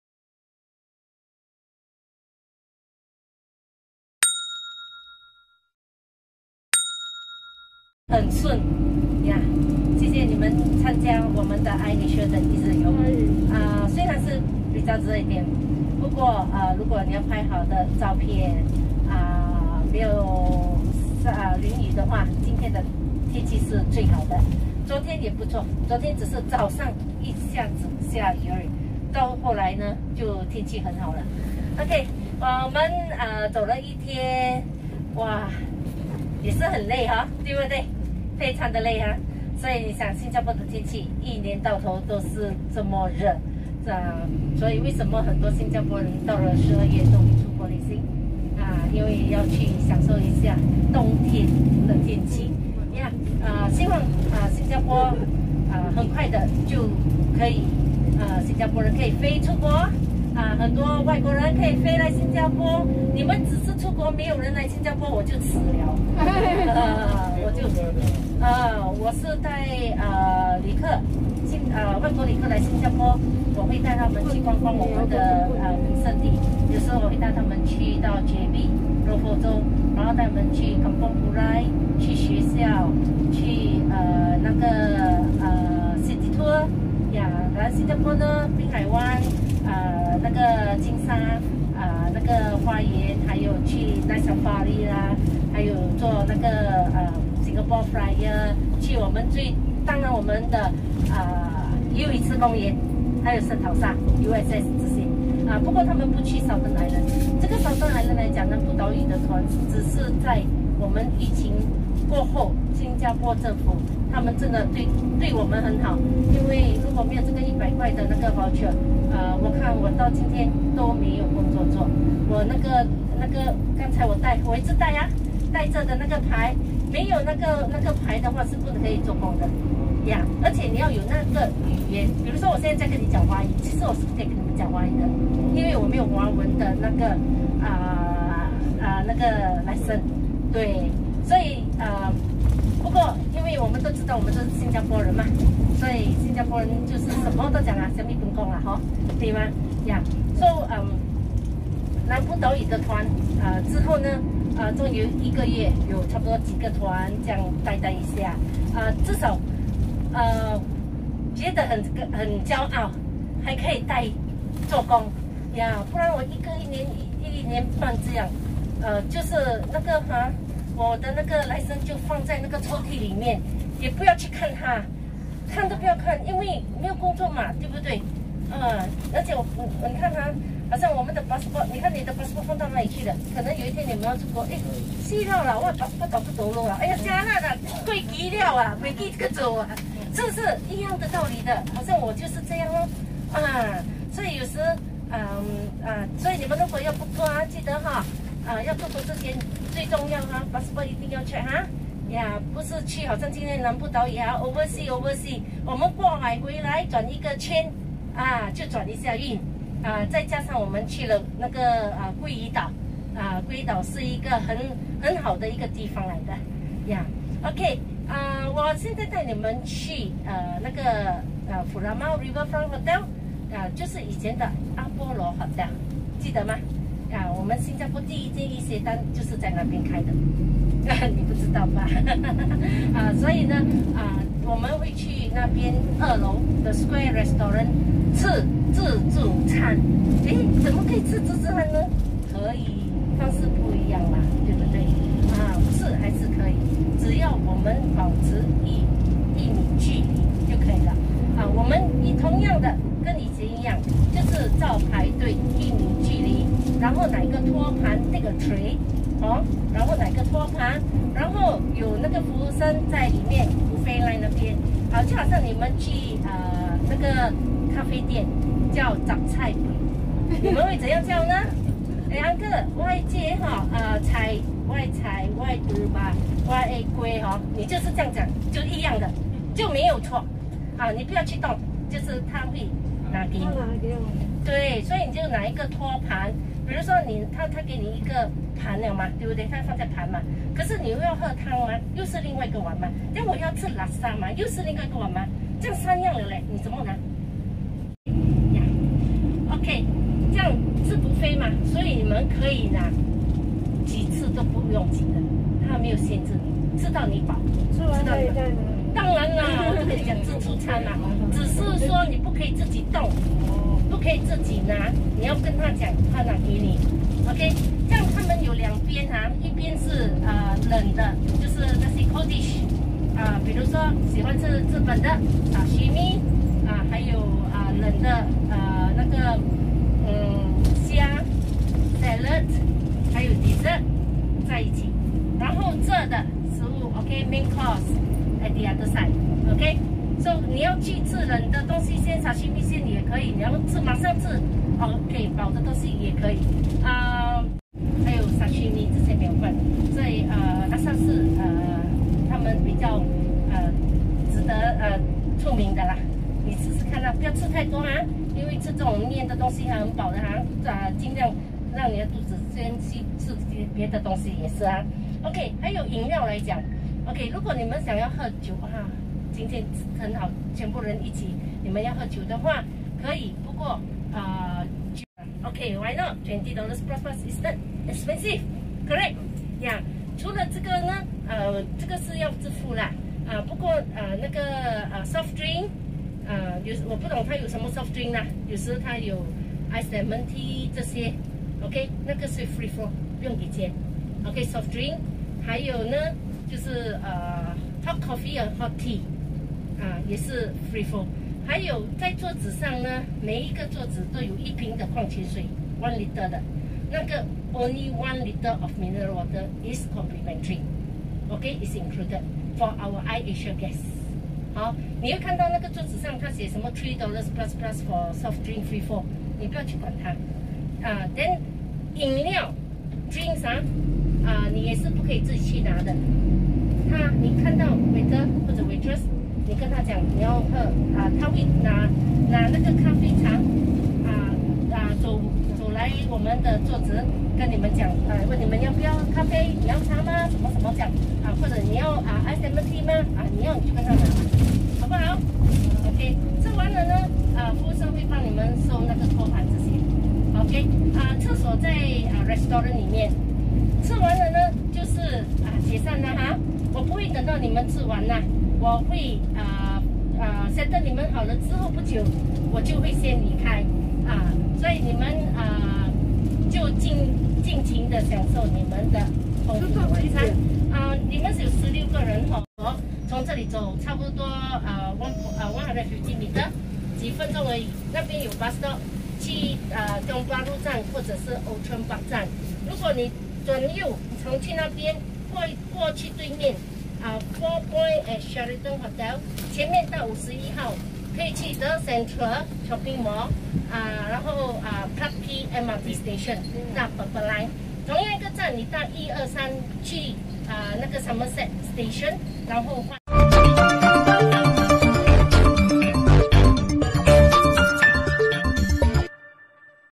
TOURIST GUIDE // MaluzSaludes
Bonding my 2nd family here in Singapore, From, marina South pier going to hotel  furama  going eating dinner